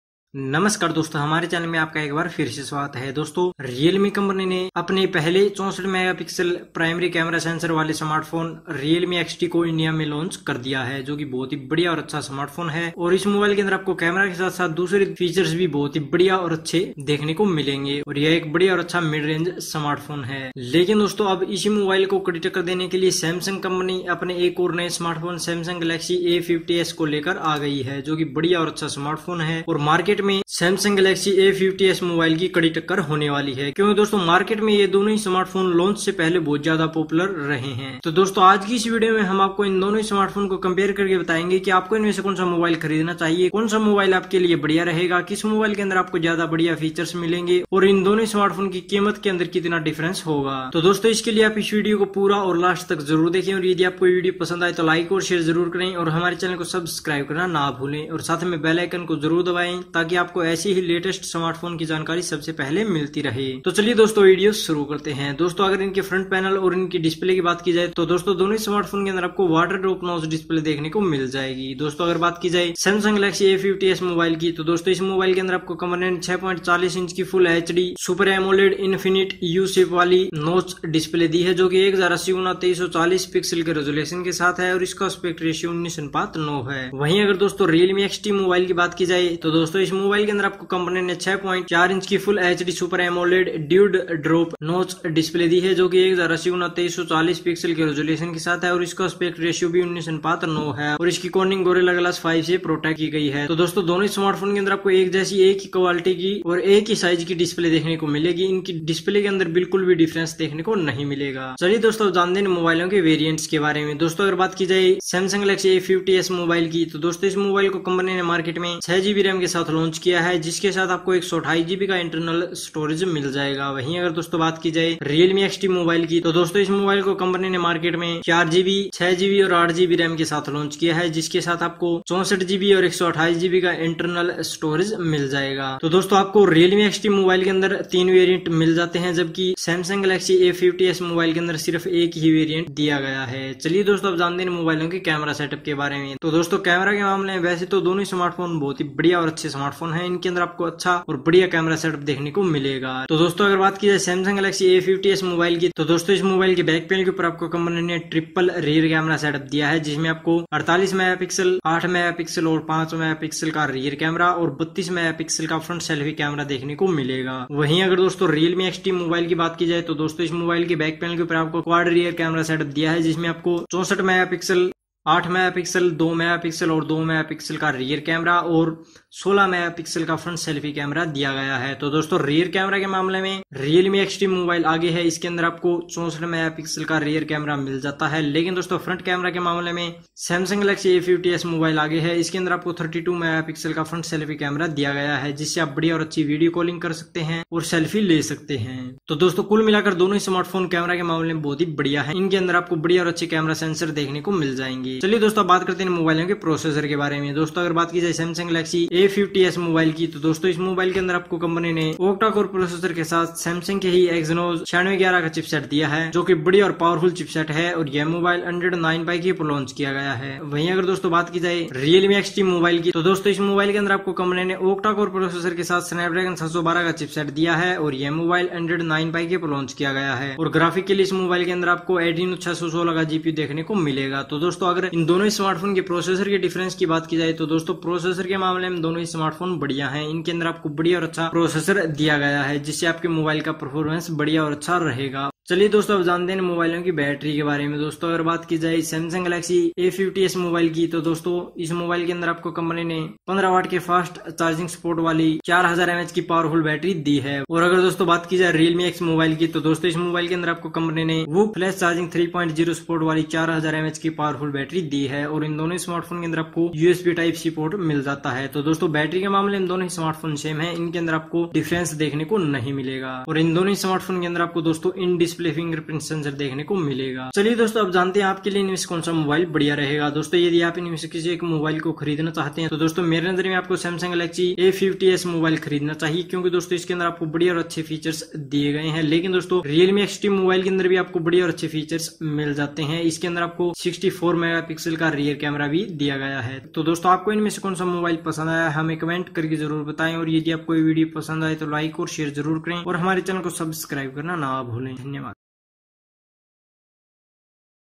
Realme XT Pro Vs Samsung Galaxy A50s Details Comparison in Hindi (2019)
Is video me aapko Realme XT Pro Vs Samsung Galaxy A50s ko compare karke bataya jayega ki konsa mobile badiya hai. Kis mobile me badiya feature diye gaye hai. Or aapko inmse konsa mobile lena chahiye. To aap is video ko pura or last tak jarur dekhe. OR yadi aapko yah video pasand aaye to like or Share jarur kare.

Realme XT

Display
6.4 inch Full HD
Resolution
1080 x 2340 pix
Aspect Ratio -19:9
Corning
Gorilla Glass

4/6/8GB RAM
64/128 GB ROM

Camera
Quad Rear Camera
64 + 8 + 2 + 2 MP
Front Camera
16 MP

Processor
Octa-core
Snapdragon 712
Android 9 Pie
GPU
Adreno 616

Battery
Non-removable Li-Po
4000 mAh battery
Vooc 3.0 Fast Charging

Display
6.4 inch HD
Super AMOLED
Resolution
1080 x 2340 pix
Aspect Ratio -19:9
Corning
Gorilla Glass

Camera
Triple Rear Camera
48 MP + 8 MP + 5 MP
Front Camera
16 MP

Processor
2.3 Octa-core
Exynos 9610
Android 9.0 (Pie)
CPU
Mali G-72

6 GB RAM
128 GB ROM

Battery
Non-removable Li-Po
4000 mAh battery
15w Fast Charging
USB
Type C - Port